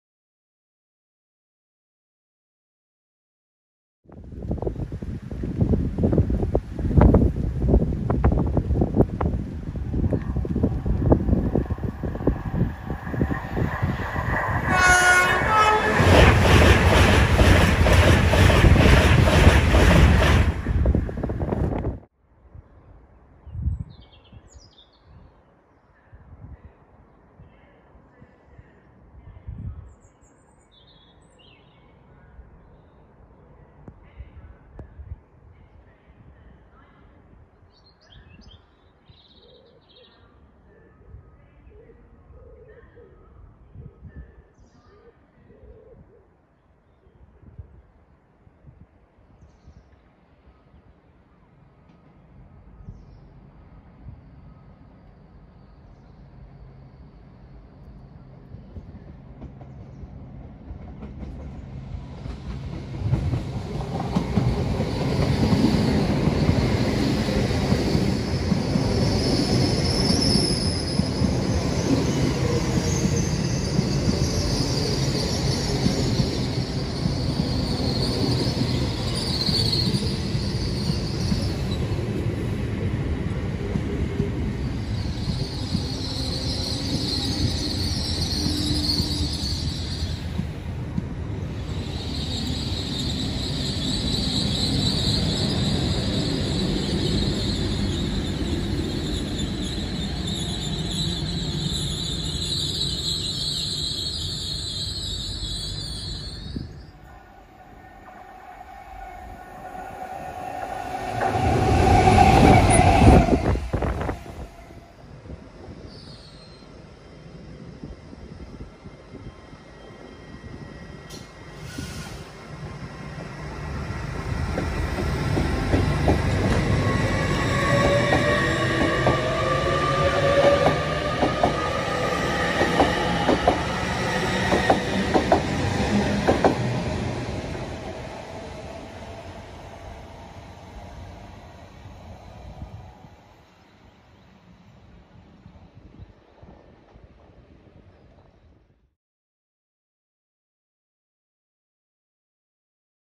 A few IETs (& Class 70) in Northumberland
Hello viewers,
In this video a trip to the Northumberland Coastline on a ‘holiday’. During that time I managed to see a few trains at different locations on the ECML! Locations include Goswick L.C, Berwick Upon-Tweed Station & Alnmouth.
Video filmed by GTR Railways Trainspotter.
Please DO NOT use without permission.
Timestamps:
0:00 Intro
0:05 LNER AZUMA Class 801218 Goswick L.C
0:22 LNER AZUMA Class 801204 Berwick Upon-Tweed
1:47 Colas Rail Freight Class 70815 Alnmouth
1:56 TPE Class 802211 Alnmouth